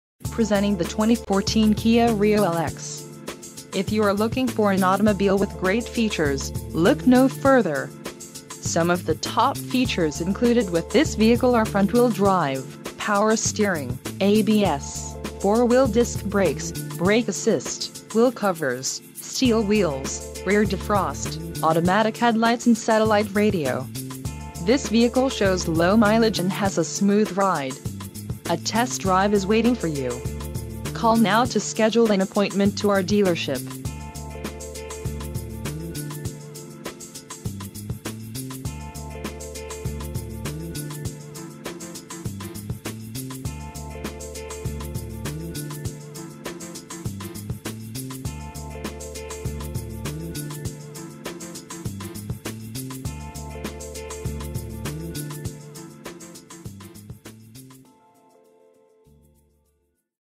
2014 Kia Rio LX - Garcia Honda - Albuquerque, NM 87110
You can find this 2014 Kia Rio LX and many others like it at Garcia Honda. The 2014 Kia offers compelling fuel-efficiency along with great value. You will no longer feel the need to repeatedly fill up this Rio LX's gas tank, in part because of it's superb EPA rating. This is about the time when you're saying it is too good to be true, and let us be the one's to tell you, it is absolutely true. At Garcia Honda, we strive to provide you with the best quality vehicles for the lowest possible price, and this Rio LX is no exception. More information about the 2014 Kia Rio: Good fuel economy and decent power are two of the main points that will ring relevant to those cross-shopping the Rio against any of the competition. In addition to being one of the lowest-priced cars in the U.S. market, the 2014 Rio is unique for being one of the few non-hybrid cars in its class to feature fuel-saving engine start/stop technology, wh... read more